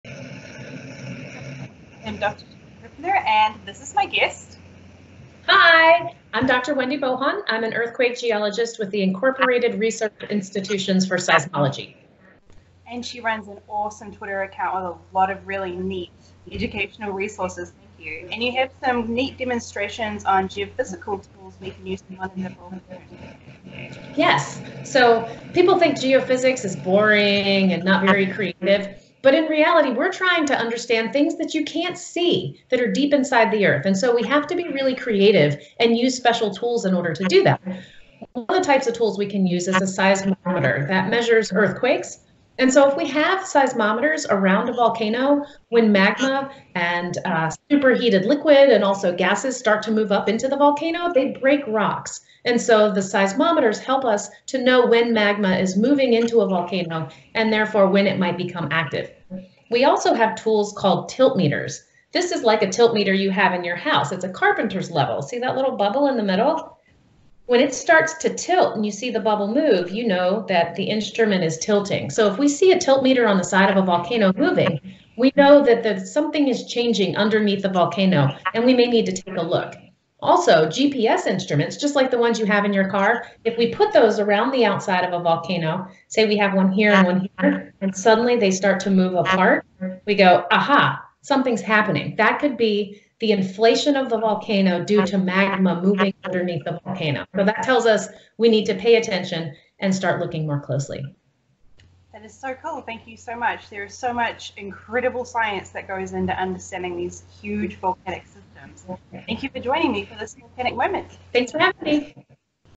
Geophysics tools for volcano monitoring
Volcano monitoring takes many tools and many experts who understand how to interpret the data of each specific method. Dr. Wendy Bohon at IRIS takes us through some geophysics tools that we use to keep an eye on volcanoes.

IRIS is a university research consortium dedicated to exploring the Earth's interior through the collection and distribution of seismographic data